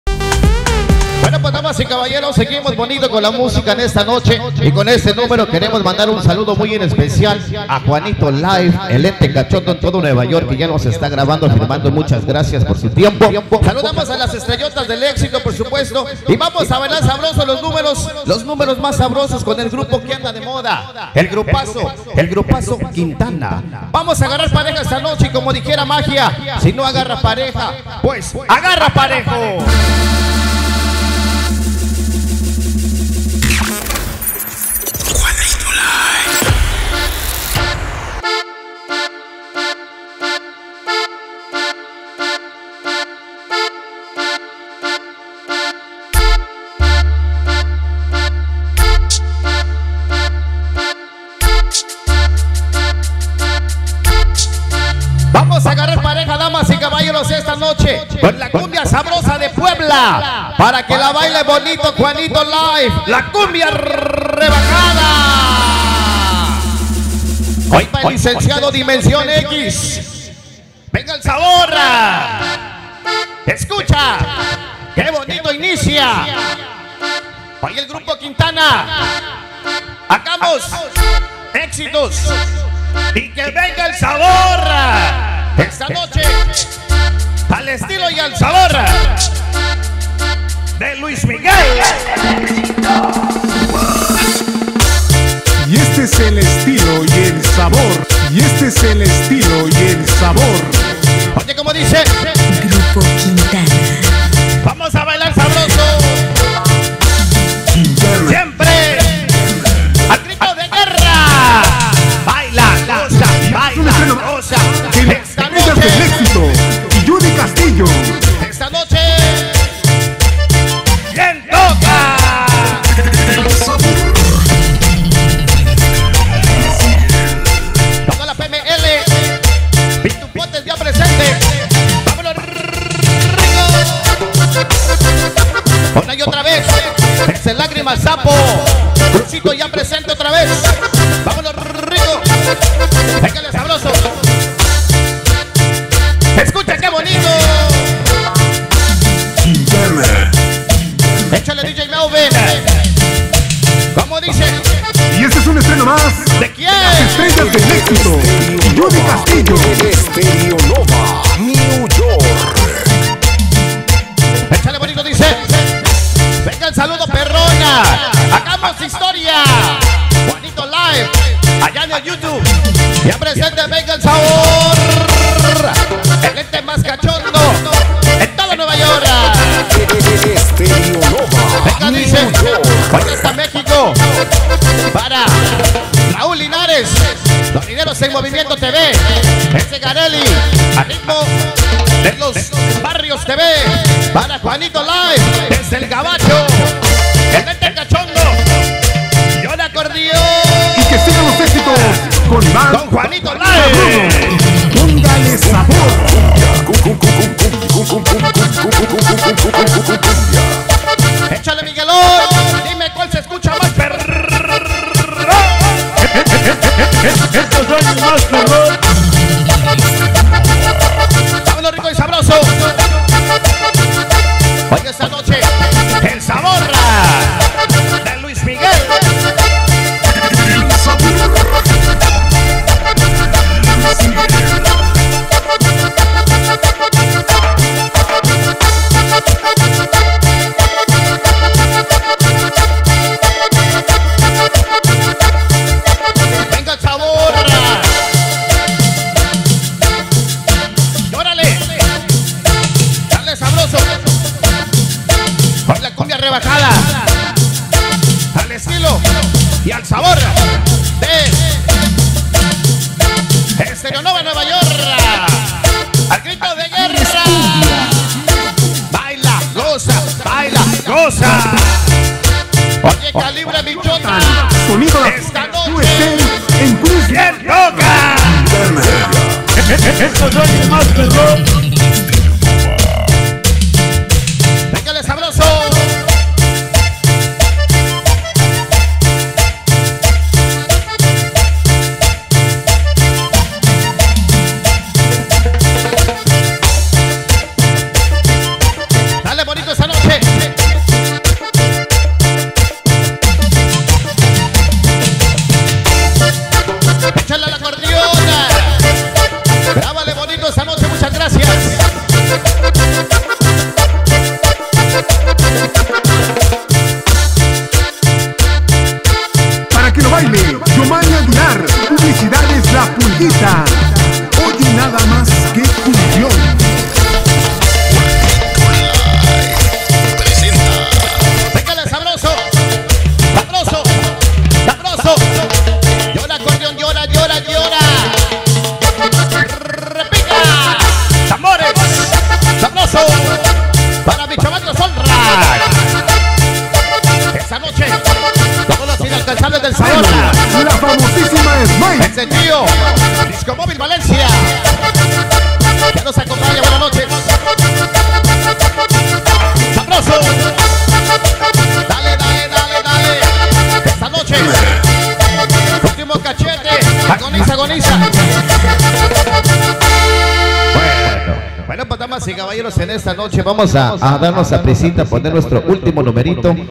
LA NUEVA CUMBIA REBAJADA 2💥22 GRUPO QUINTANNA EN VIVO SONIDO ESTEREO NOVA CLUB SL LOUNGE QUEENS NY
ESTRENO 2💥22 LA CUMBIA REBAJADA ~ GRUPO QUINTANNA CON SONIDO ESTEREO NOVA - CLUB SL LOUNGE QUEENS NY🗽